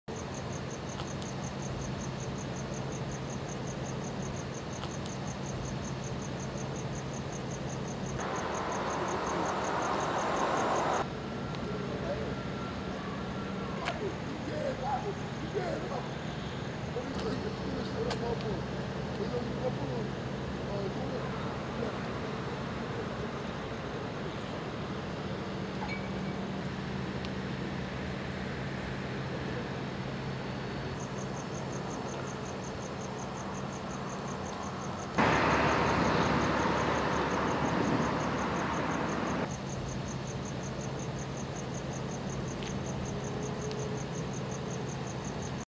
Police: Man shot in torso during robbery in Jacksonville
Police say the man was taken to the hospital and will recover.